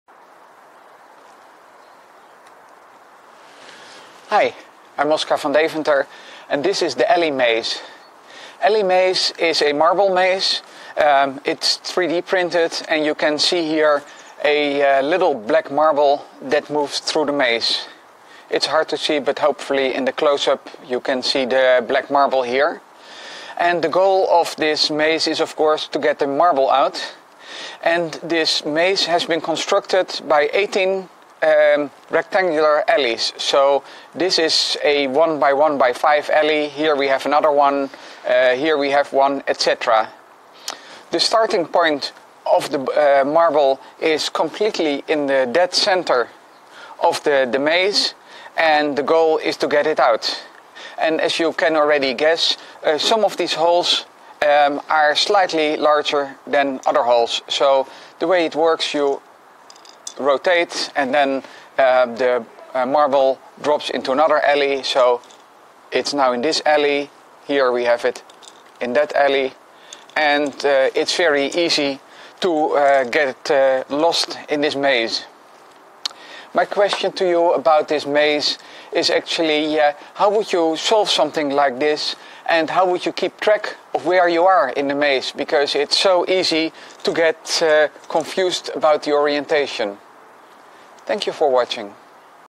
Alley Maze - When did you get lost in an alley?
Alley Maze is a ball maze. The object is to get the 8-mm ball out. The puzzle consist of eighteen 1x1x5-unit alleys that are put together into a cube. Some of the alleys are connected via larger holes, so the ball can pass through. The ball starts from the middle of the cube. The maze pattern is "interconnected loops". So when solving the maze, one may go around a loop several times, before accidentially passing to a next loop. Or to a previous one.